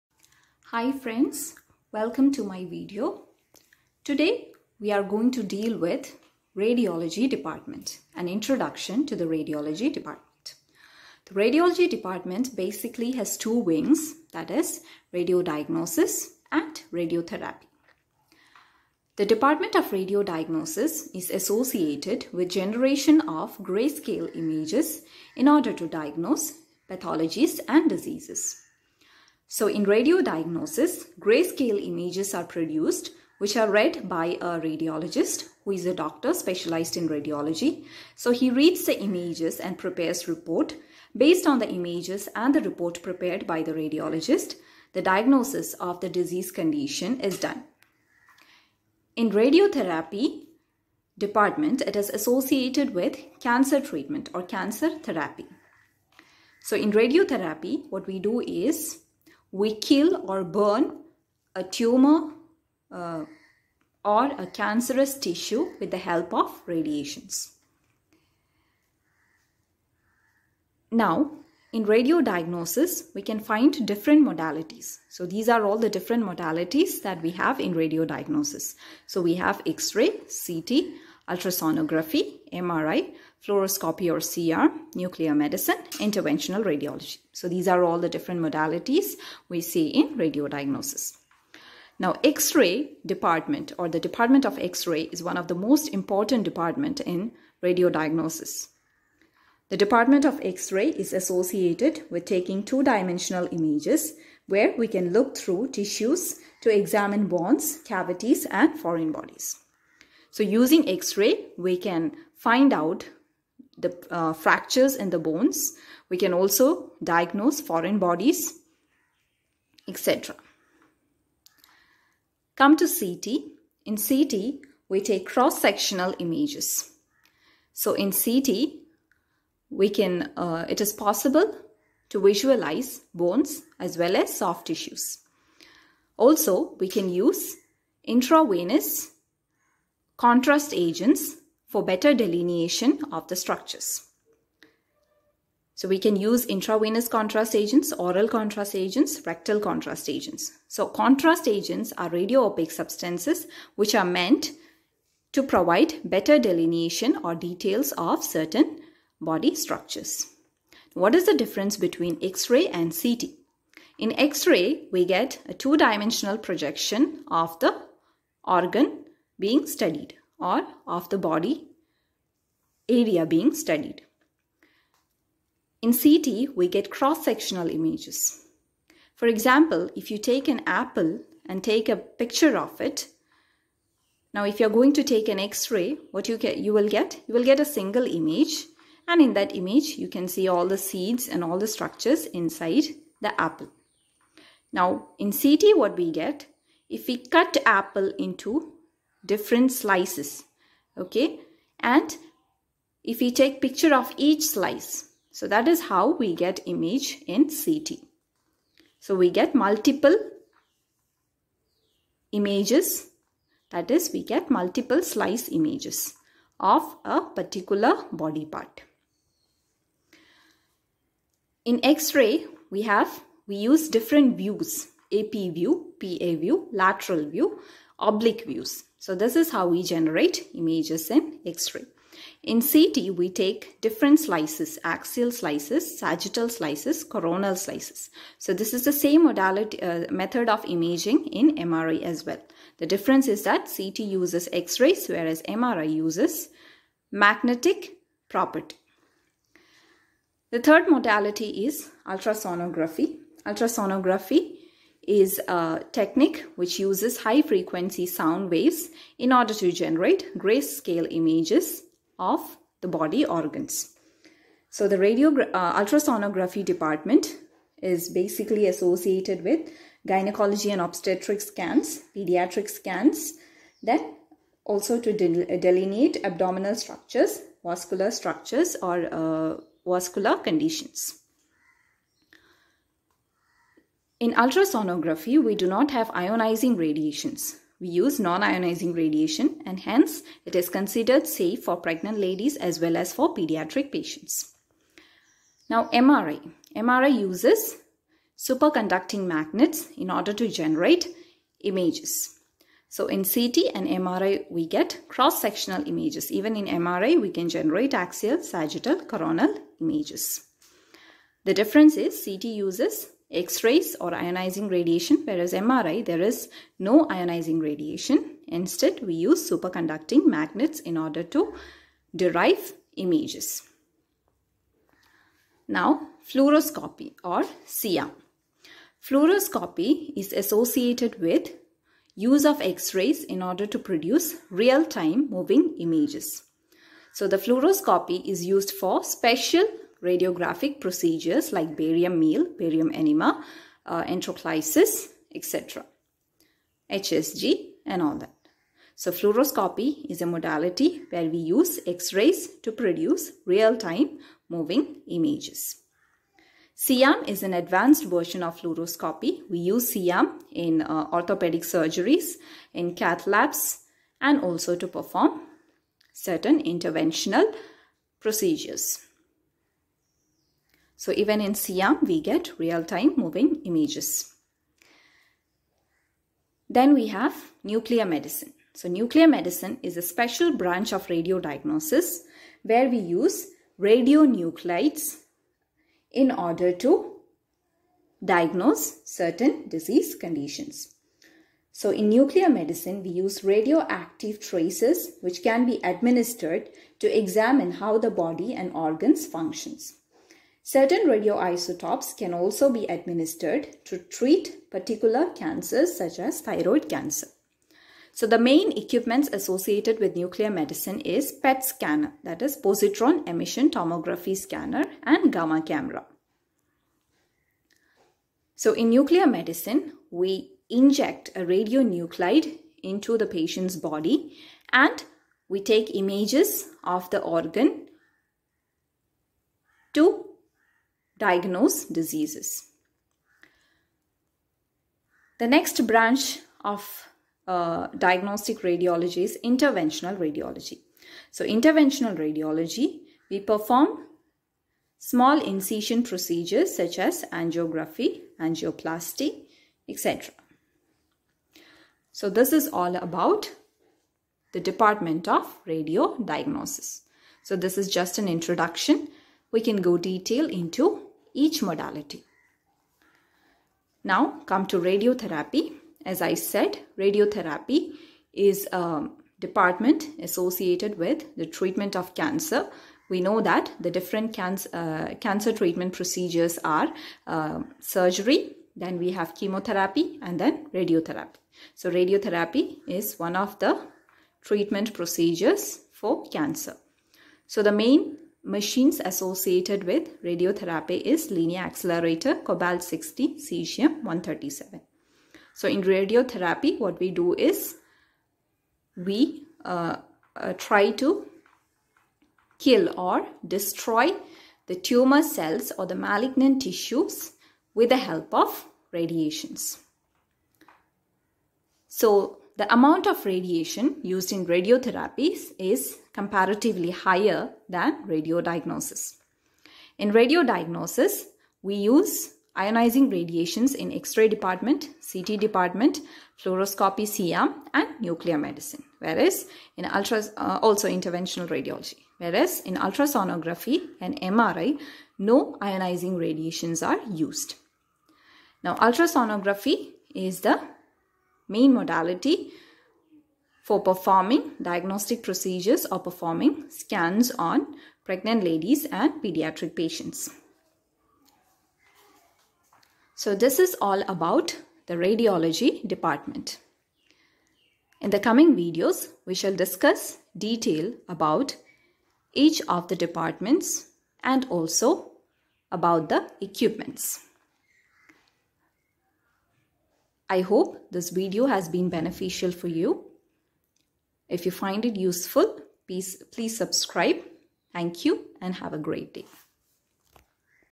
Department of radiology in hospital setting explained
Radiology - an overview
x-ray, CT, MRI, Nuclear Medicine, radiotherapy Nimisha Martina James# Radiology# Educational video